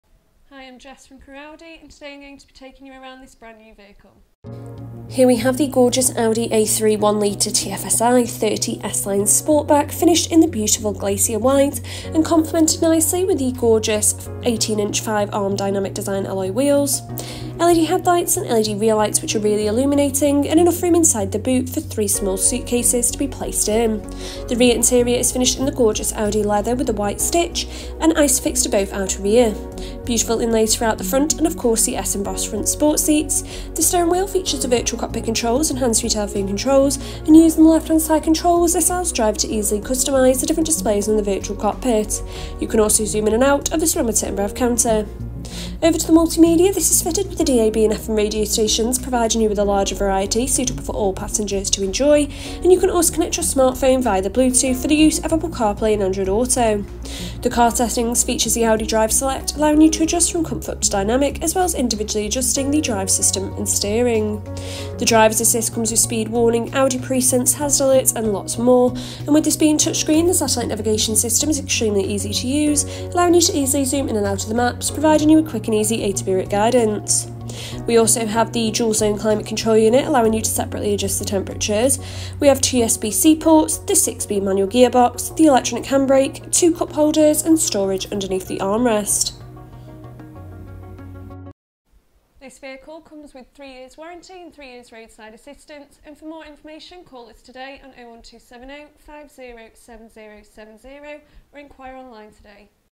Brand New - Audi A3 1.0 TFSI 30 S line Sportback Euro 6 (s/s) 5dr - Crewe Audi
Audi A3 1.0 TFSI 30 S line Sportback Euro 6 (s/s) 5dr

Colour : Glacier White
Fuel : Petrol
Transmission : Manual